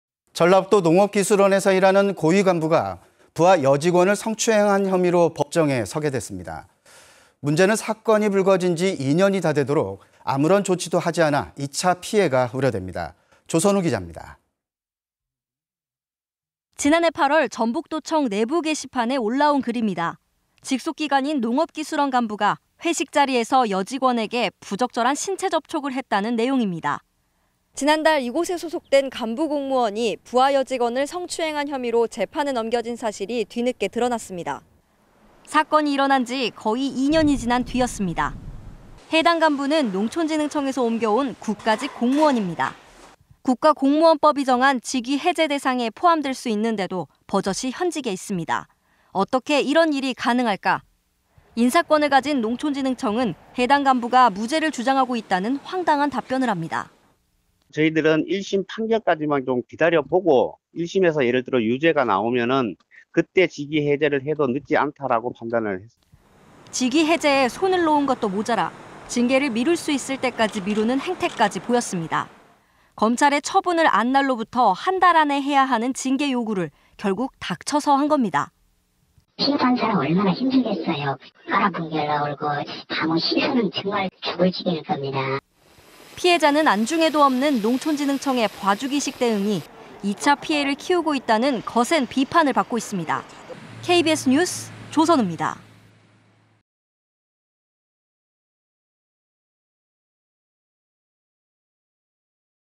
여직원 추행 고위 공무원 '봐주기 일관' 2019.05.15(수)
[앵커]

전라북도 농업기술원 고위 간부가
부하 여직원을 추행한 혐의로 법정에 서게 됐는데요.
사건이 불거진 지 2년이 다 되도록,
피해 직원과 한 건물에서 일하는
황당한 일이 벌어지고 있습니다.

조선우 기자입니다.

[리포트]

전북도청 내부 게시판에
올라온 글입니다.

직속 기관인 농업기술원 간부가 회식 자리에서
여직원에게 부적절한 신체 접촉을 했다는 내용입니다.

지난달 이곳에 소속된 간부 공무원이
부하 여직원을 성추행한 혐의로
재판에 넘겨진 사실이 뒤늦게 드러났습니다.

사건이 일어난 지,
거의 2년이 지난 뒤였습니다.

해당 간부는 농촌진흥청에서 옮겨온
국가직 공무원입니다.

국가공무원법이 정한
직위 해제 대상에 포함될 수 있는데도
버젓이 현직에 있습니다.

어떻게 이런 일이 가능할까.

인사권을 가진 농촌진흥청은
해당 간부가 무죄를 주장하고 있다는
황당한 답변을 합니다.

심규선 / 농촌진흥청 인사팀장
"저희는 1심 판결까지만 좀 기다려보고 1심에서 예를 들어 유죄가 나오면 그 때 직위 해제를 해도 늦지 않다라고 판단을.."

직위 해제에 손을 놓은 것도 모자라,
징계를 미룰 수 있을 때까지 미루는
행태까지 보였습니다.

검찰의 처분을 안 날로부터
한 달 안에 해야 하는 징계 요구를
결국, 닥쳐서 한 겁니다.

전라북도 농업기술원 직원(음성변조)
"피해받은 사람은 얼마나 힘들겠어요. / (위에서) 깔아뭉개려고 하고 시선은 정말 / 죽을 지경일 겁니다."

피해자는 안중에도 없는
농촌진흥청의 봐주기식 대응이
2차 피해를 키우고 있다는
거센 비판을 받고 있습니다.

KBS뉴스 조선우입니다.